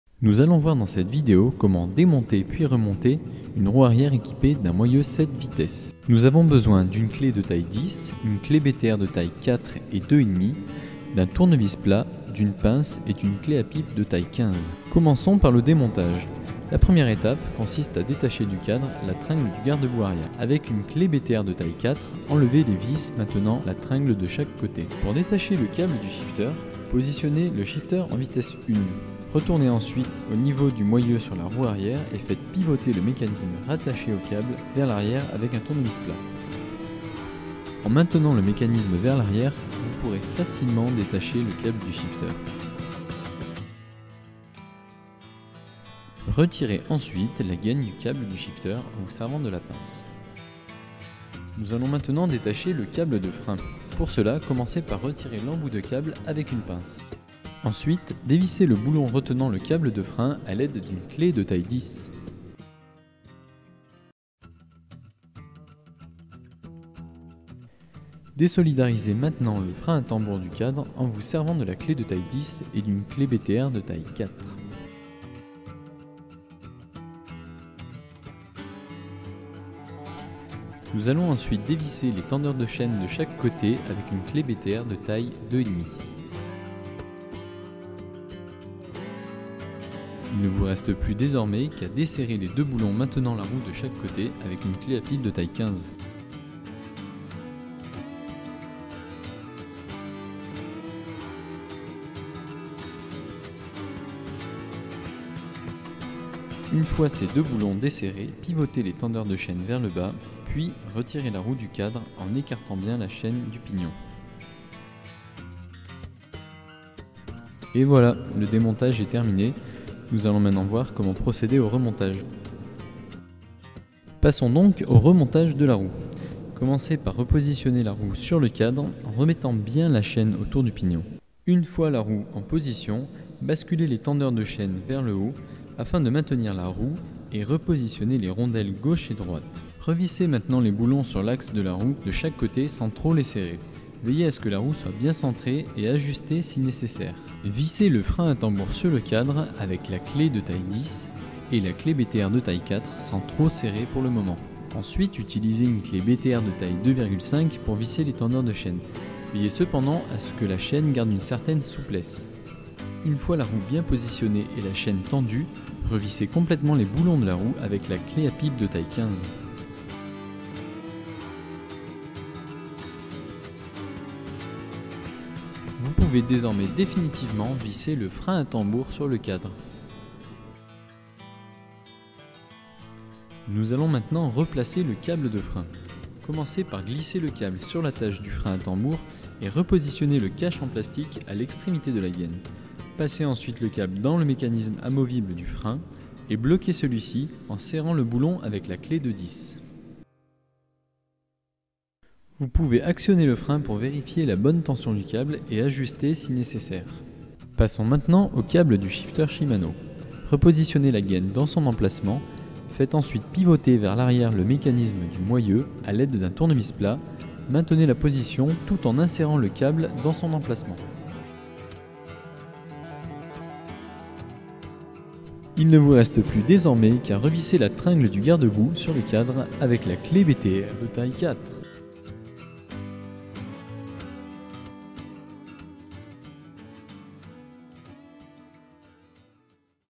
United Cruiser - Démontage de roue arrière Nexus 7i
United Cruiser vous accompagne étape par étape dans le démontage et montage d'une roue arrière de vélo équipé d'un moyeu shimano Nexus 7i.